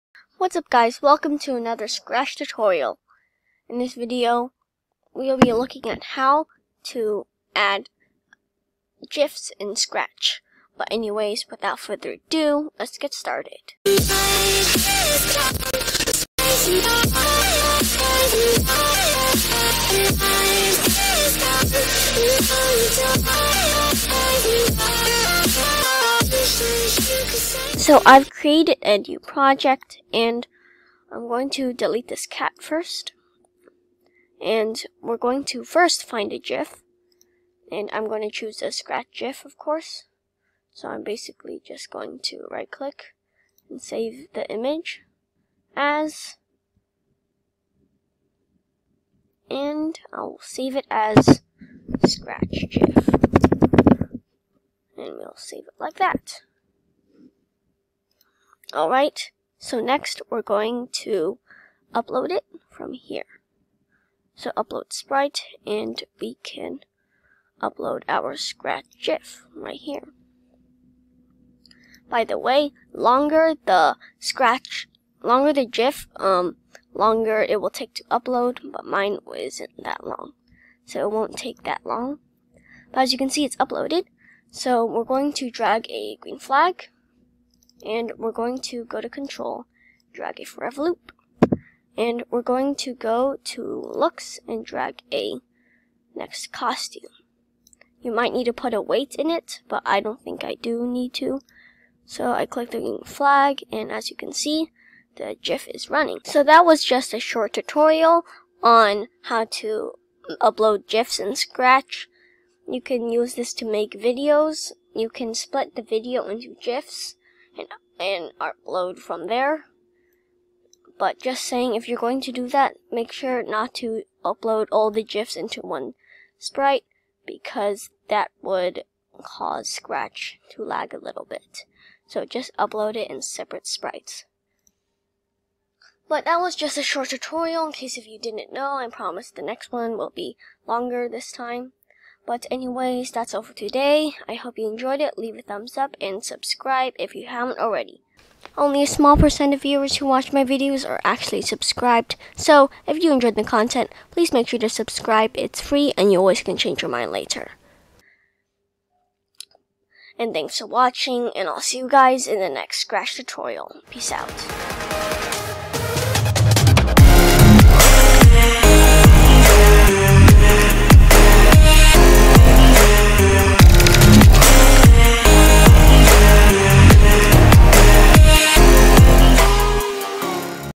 How to Upload Gifs on Scratch
Hey guys! In this video we'll be looking at how to upload gifs on scratch. You can use this to make videos and other things on scratch!